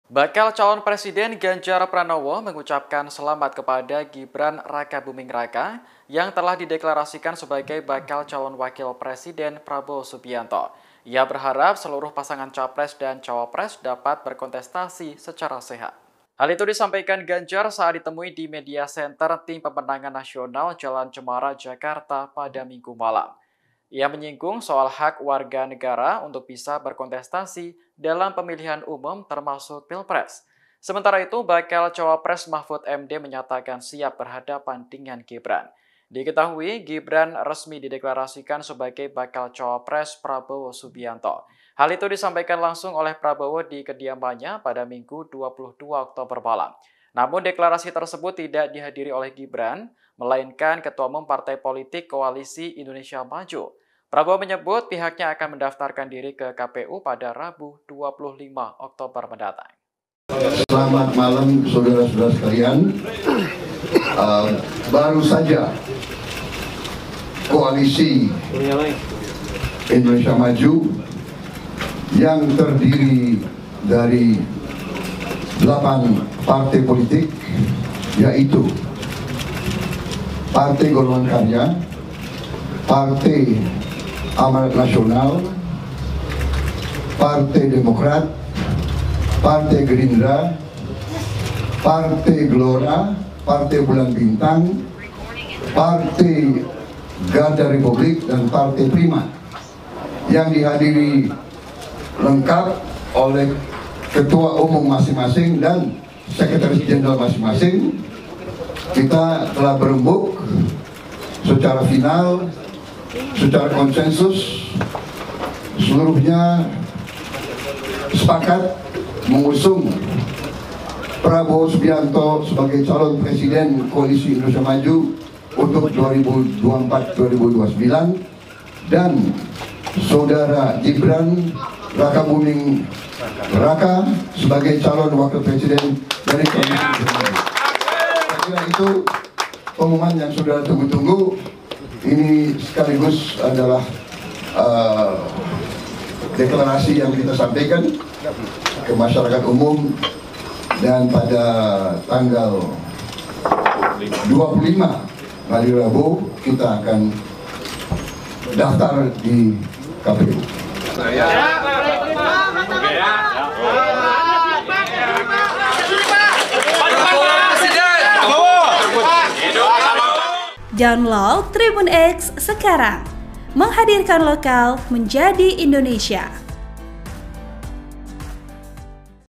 Prabowo Gibran Resmi, Ganjar Mahfud Minta Tanding Fair di Pilpres 2024
Bakal calon presiden Ganjar Pranowo mengucapkan selamat kepada Gibran Rakabuming Raka yang telah dideklarasikan sebagai bakal calon wakil presiden Prabowo Subianto.

Ia berharap, seluruh pasangan capres-cawapres dapat berkontestasi secara sehat.